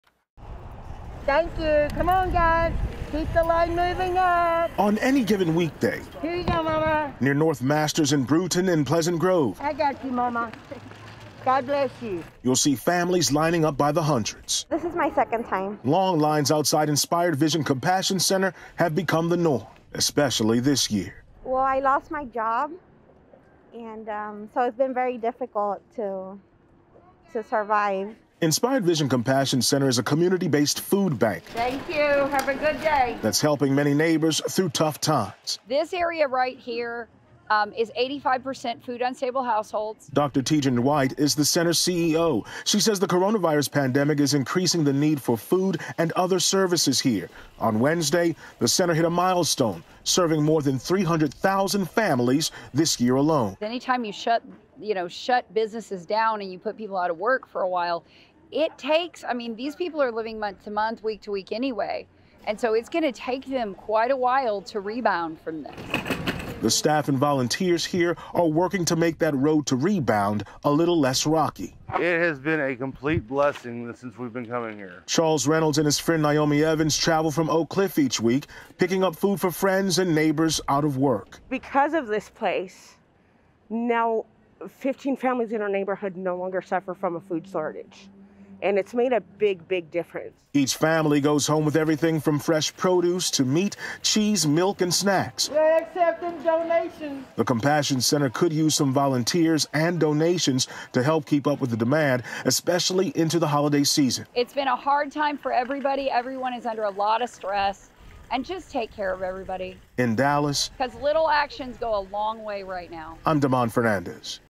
Dallas food bank serves record 300,000 families during pandemic
Hundreds of families line up at Inspired Vision Compassion Center every weekday for food and other resources.